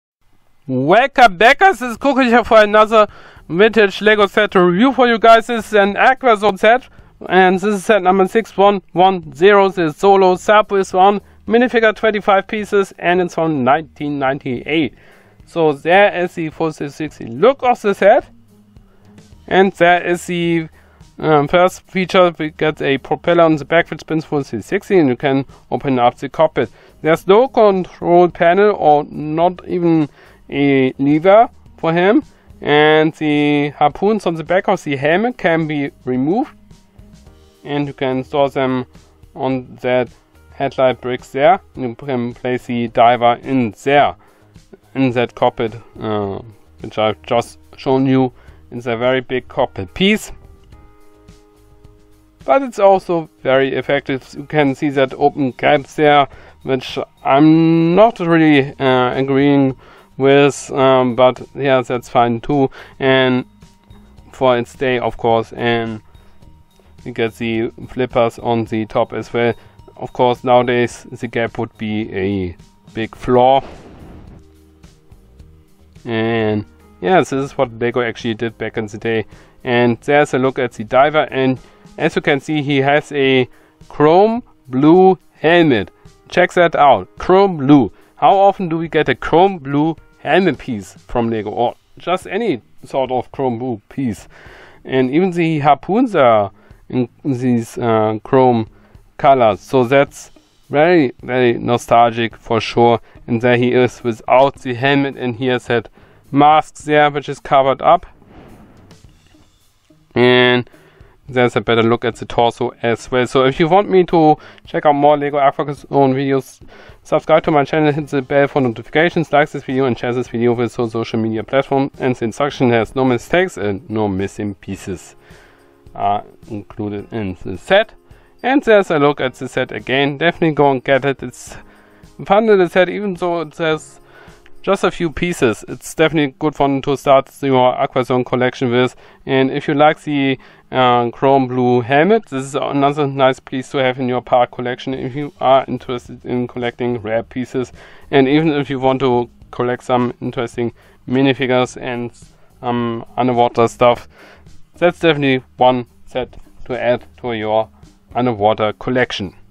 LEGO Aquazone Solo Sub Set 6110 Review
This is my review of LEGO® set 6110 Solo Sub from LEGO® Aquazone...

Informations:

Name: Solo Sub
Number: 6110
Theme: Aquazone
Minifigures: 1
Pieces: 26
Released: 1998

I bought this product. All opinions are my own.